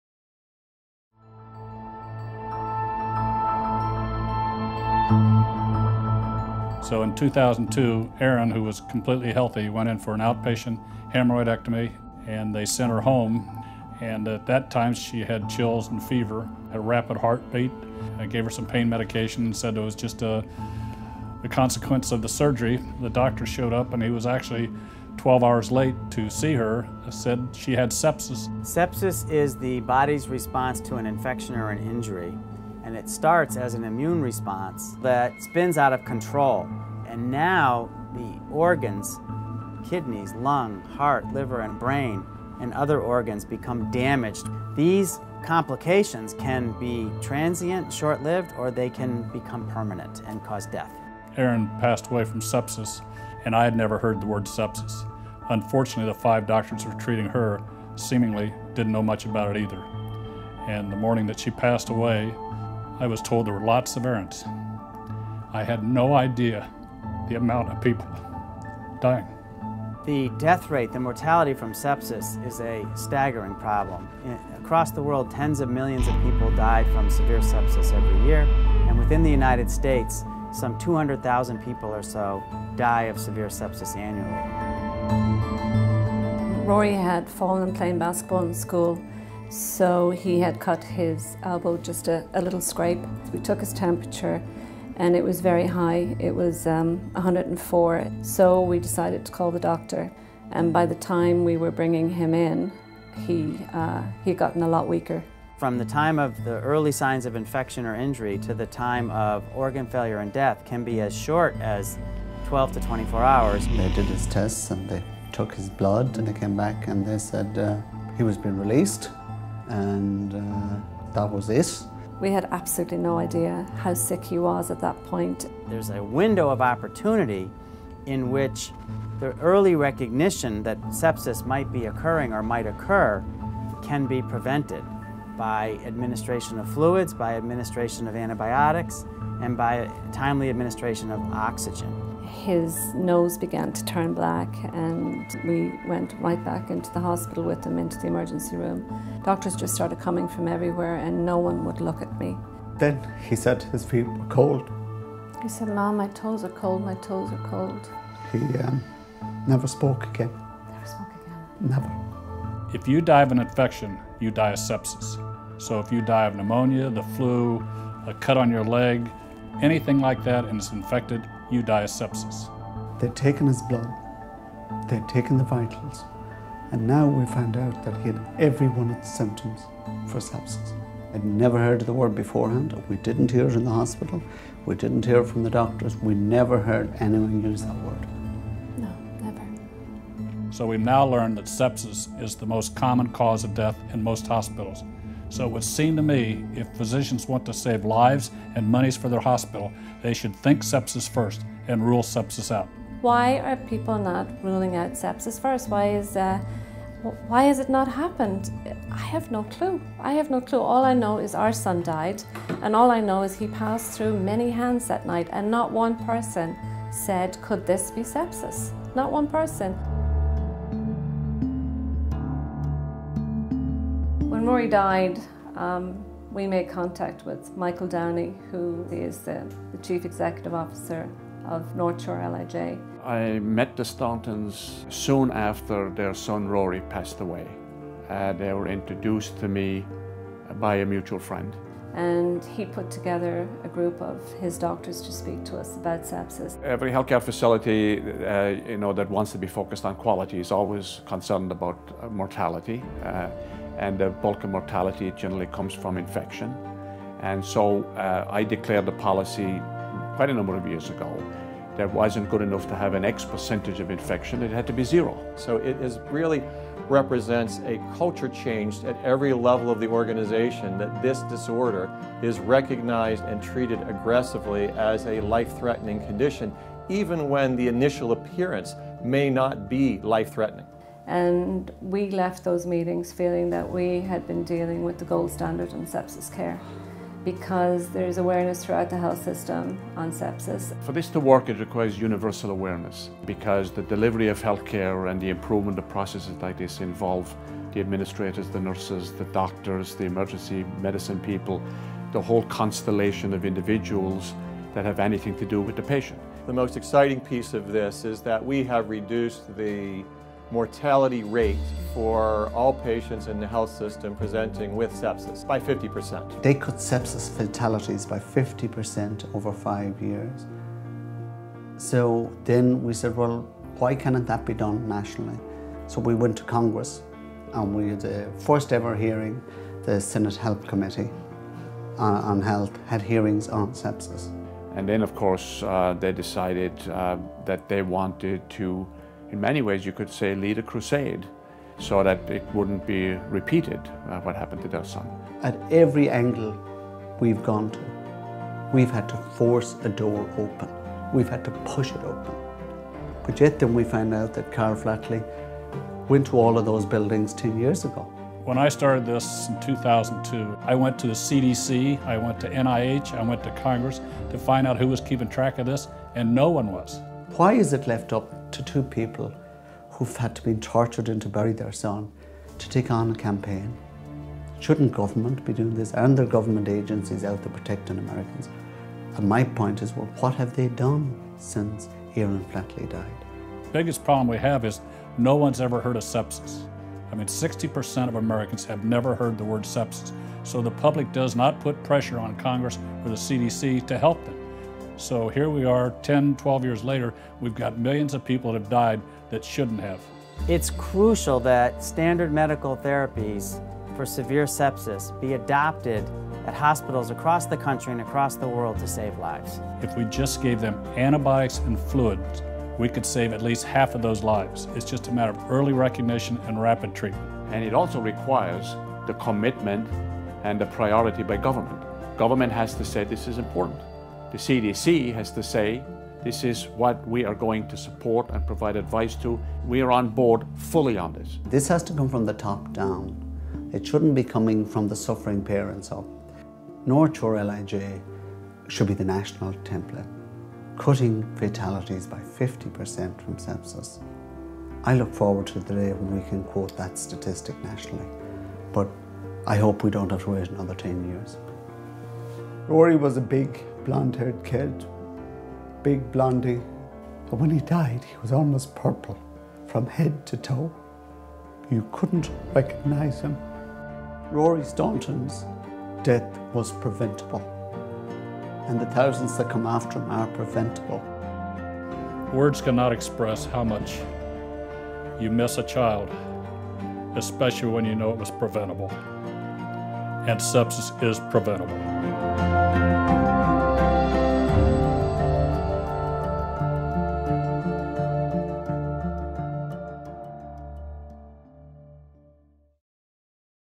Rory Staunton Foundation Video
The Rory Staunton Foundation supports education and outreach efforts aimed at rapid diagnosis and treatment of sepsis, particularly in children.